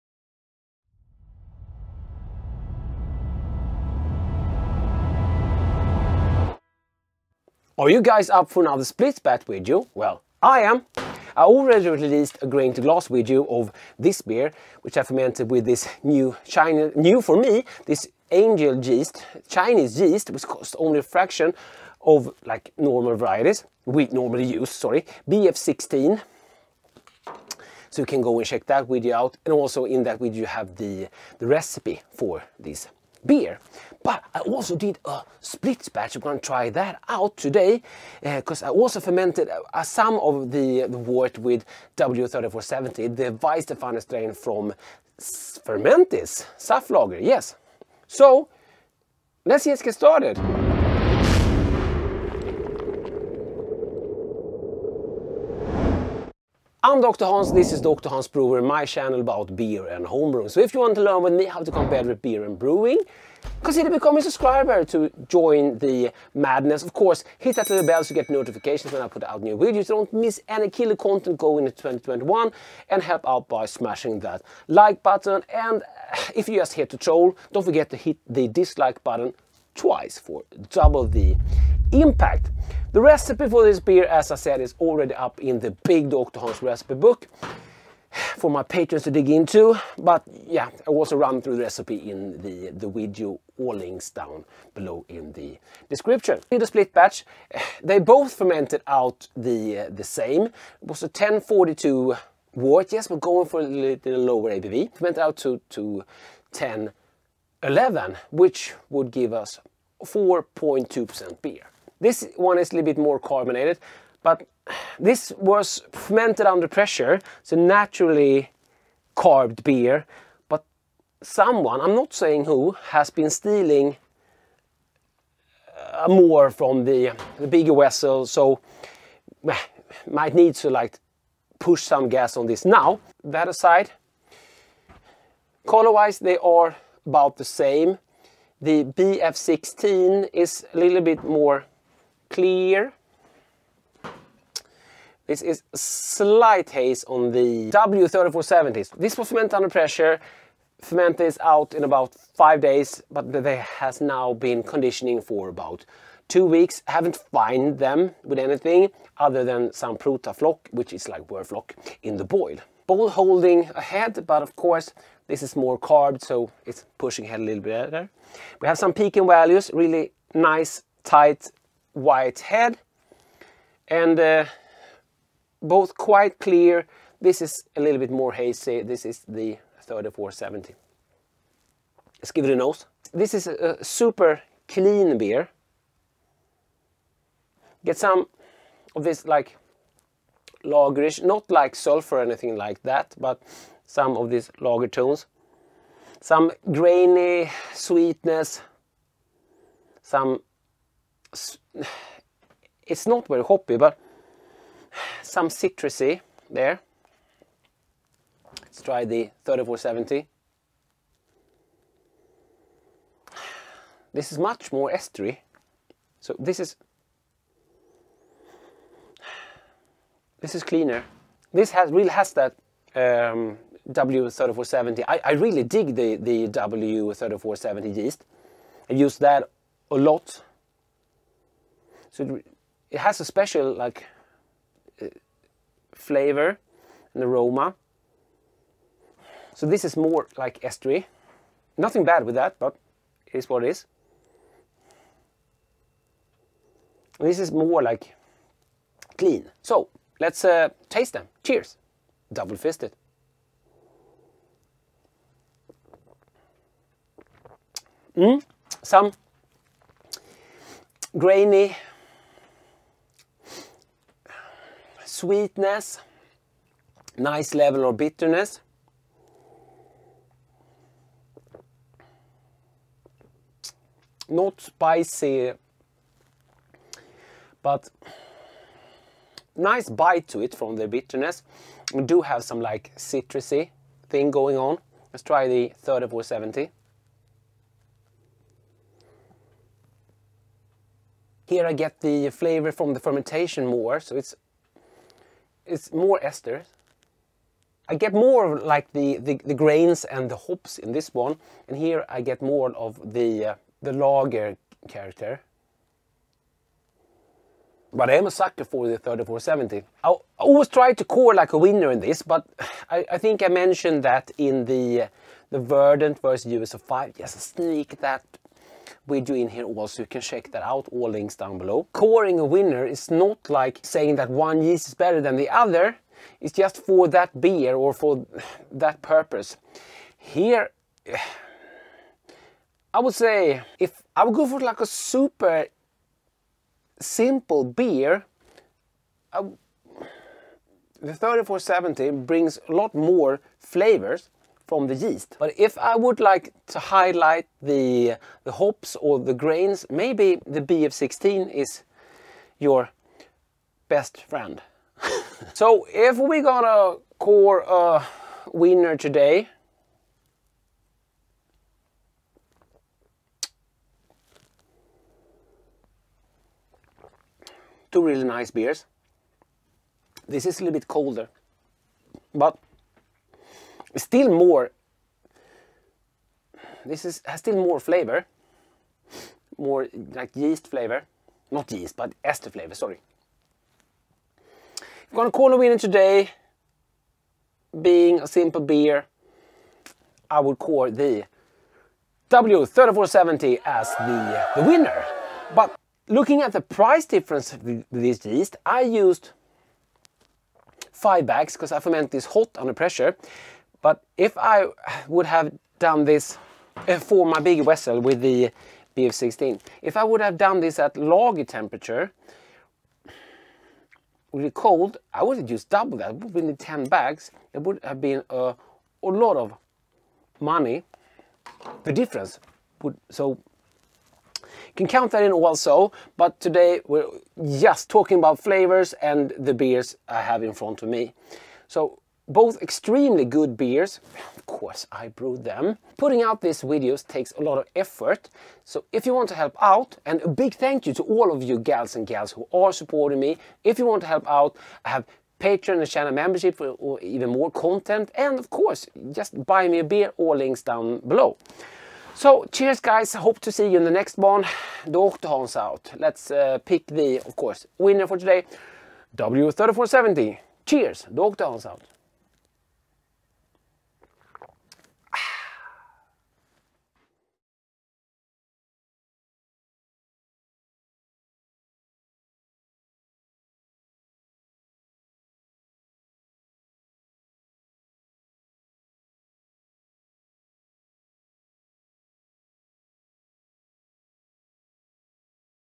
Angel Yeast BF16 VS Saflager w34/70 - Split Batch Experiment
Split batch experiment!
BF16 vs US05 yeast

Liked the video?